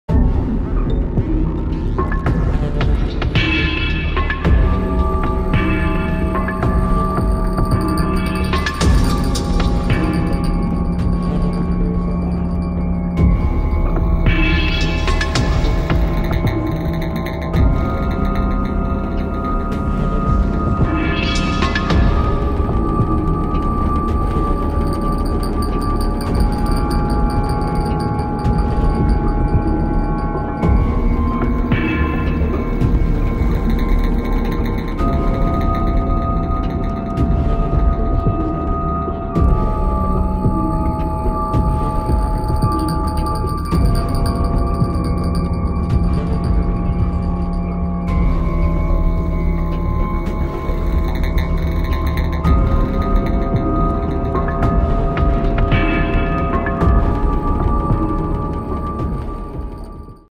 SubSpecies - The End Games Audio: The Lab
A clip from Sub Species - The End Games that was part of Alton Towers' Scarefest 2015.